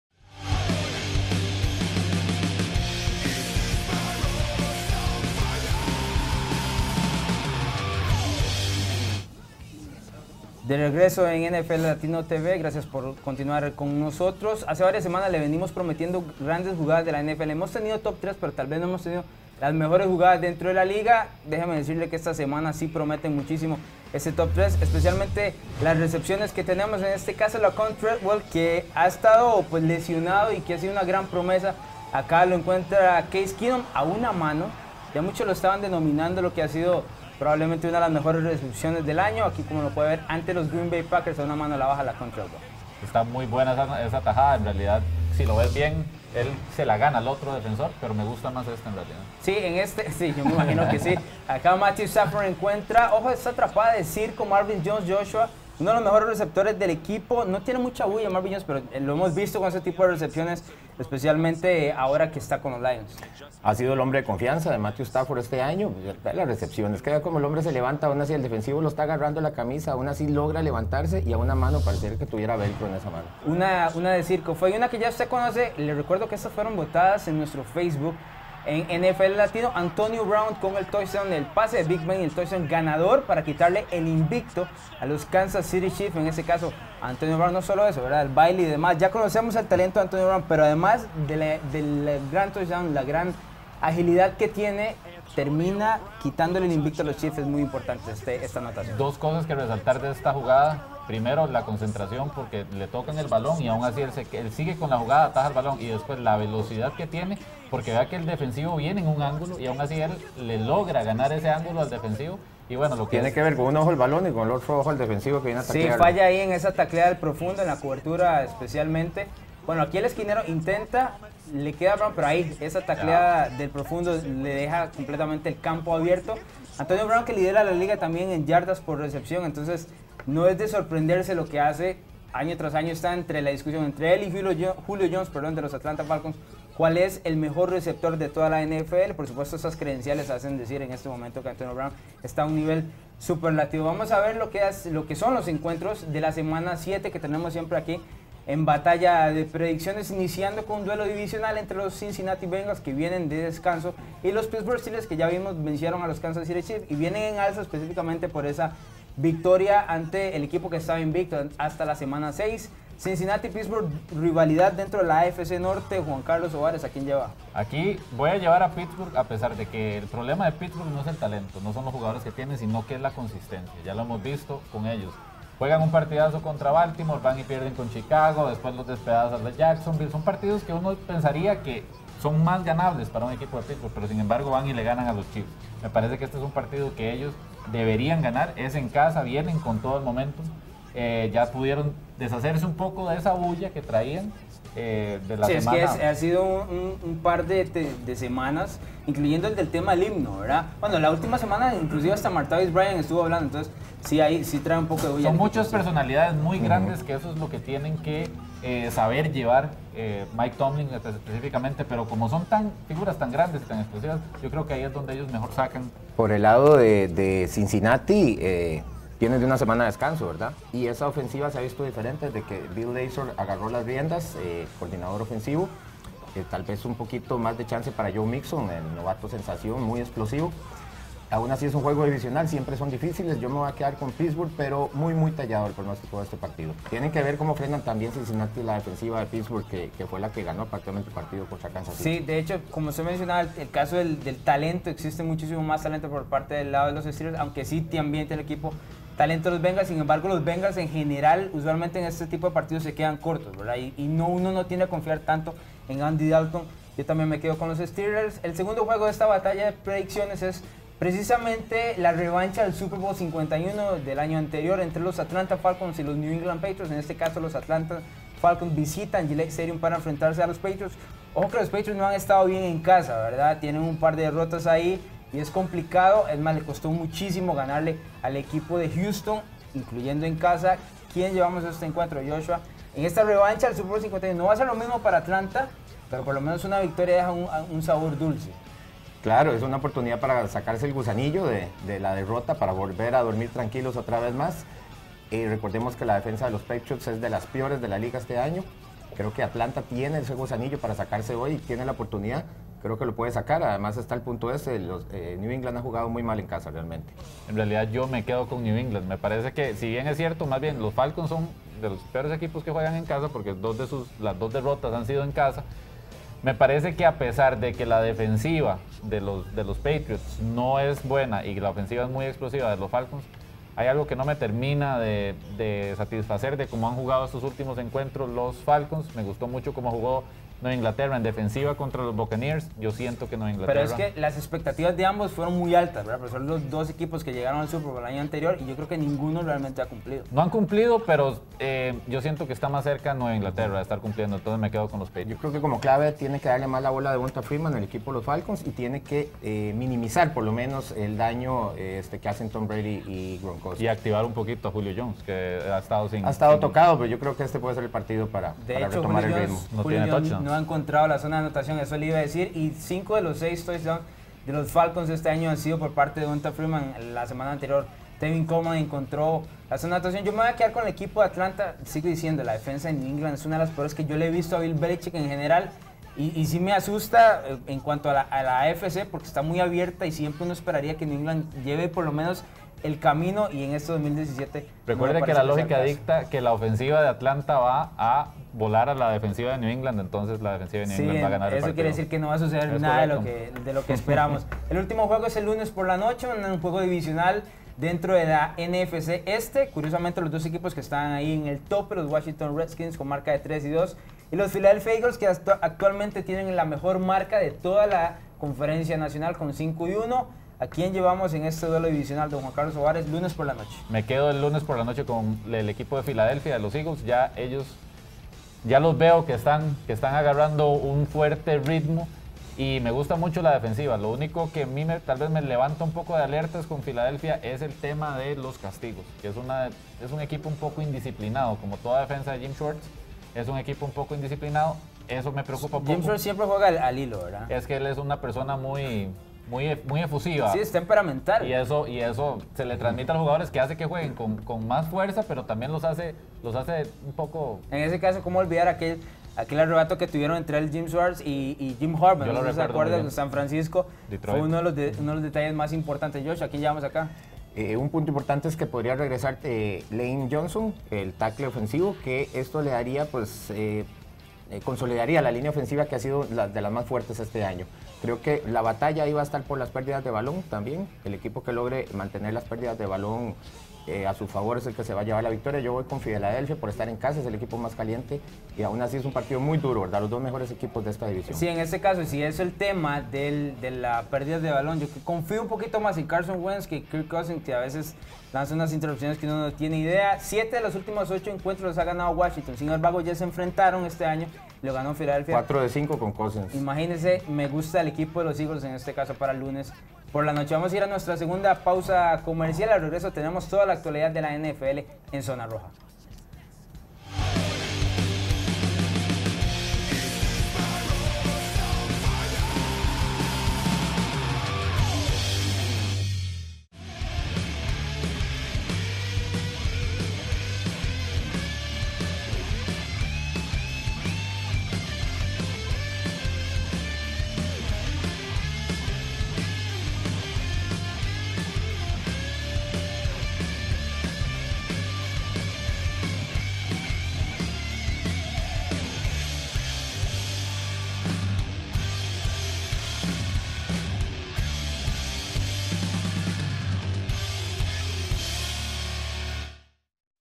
Top 3 Y Batalla de Predicciones Rumbo A La Semana 7
Predecimos el Bengals vs Steelers
Falcons vs Patriots
Redskins vs Eagles
de la Semana 7